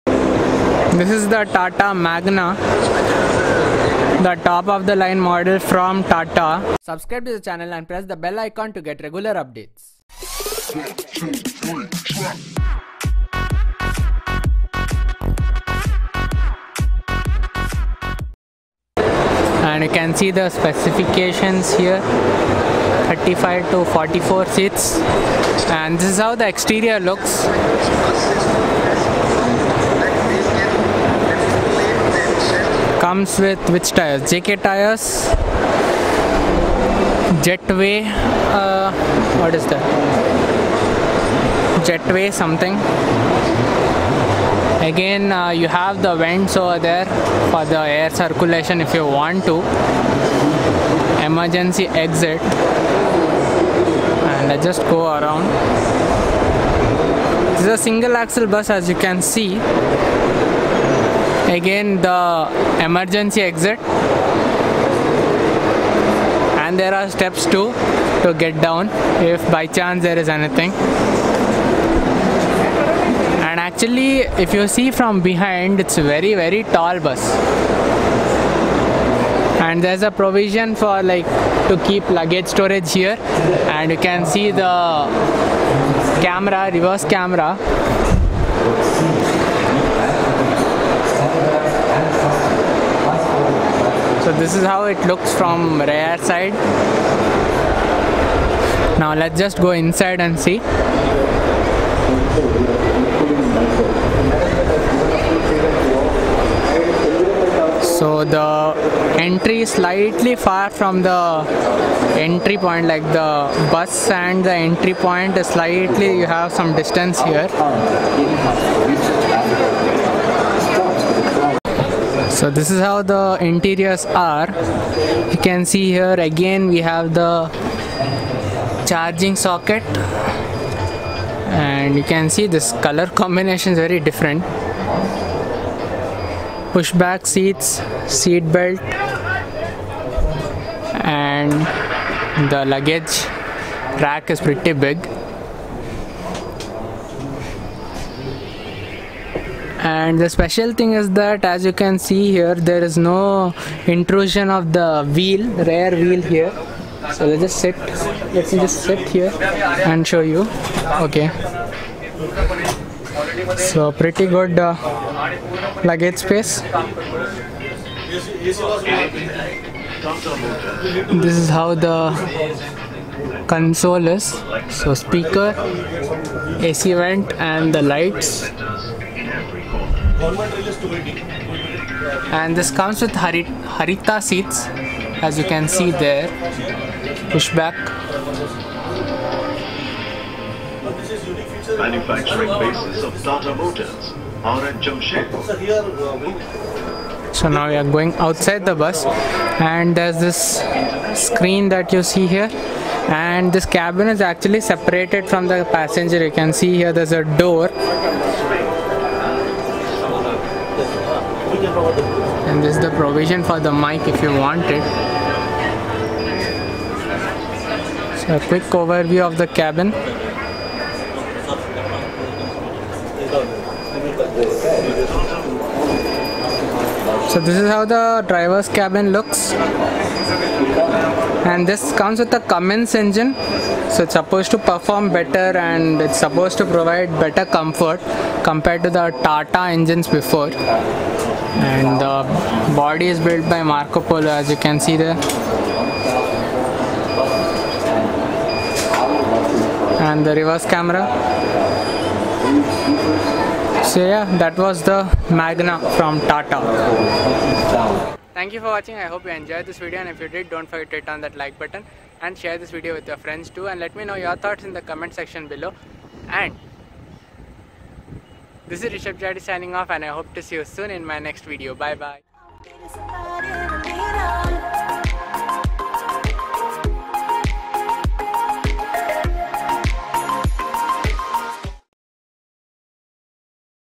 Premium Bus from Tata Marcopolo Magna AC Semi SLeeper BusWorld 2018
I hope you enjoy this video and if you do then please click on the like button and share the video with your friends too .